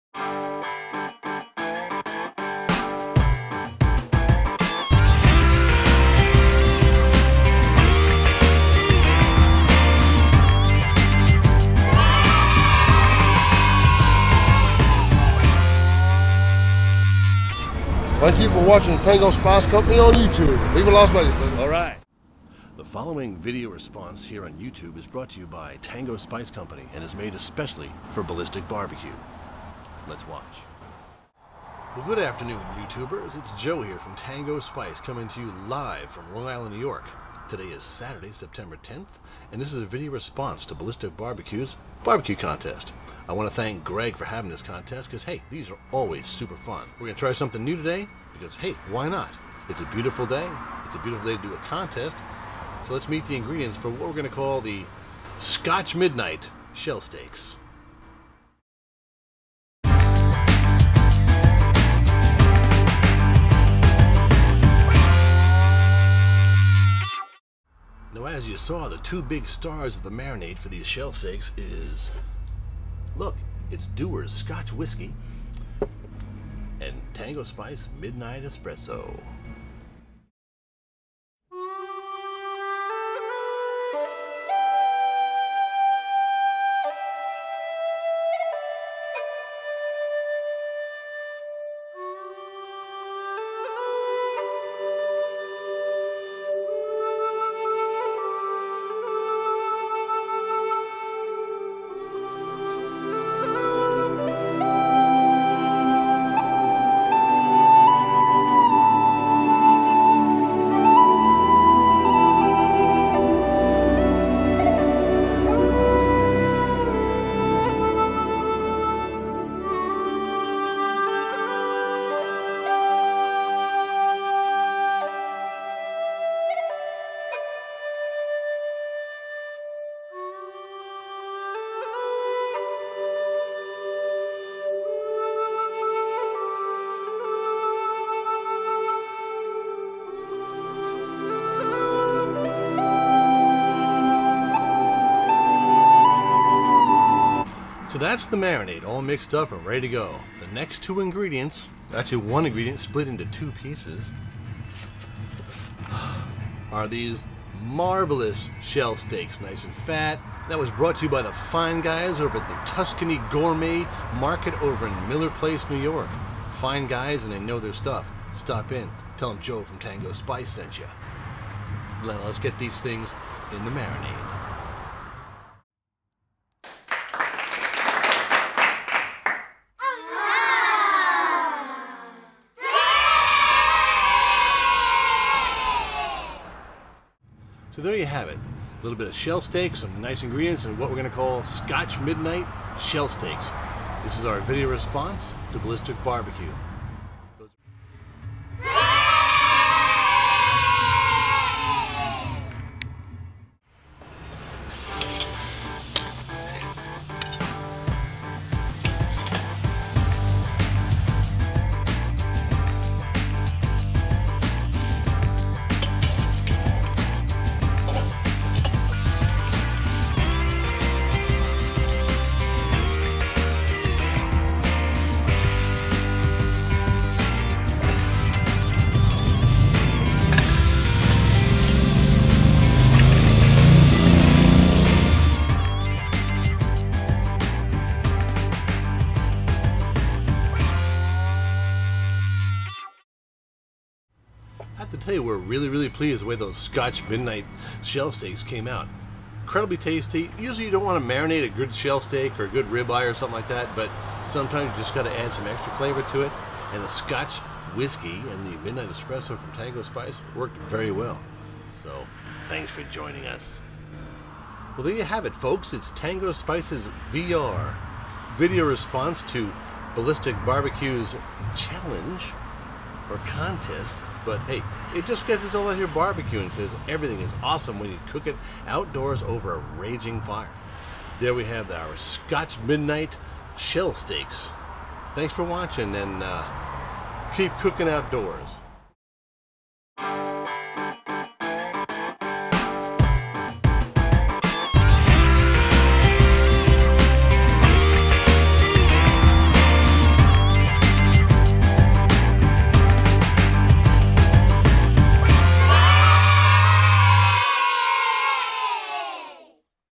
Scotch Midnight Shell Steaks
The Following Is A Video Response For Ballistic BBQ's Grilling Contest!

You can read the recipe on the Tango Test Kitchen Blog.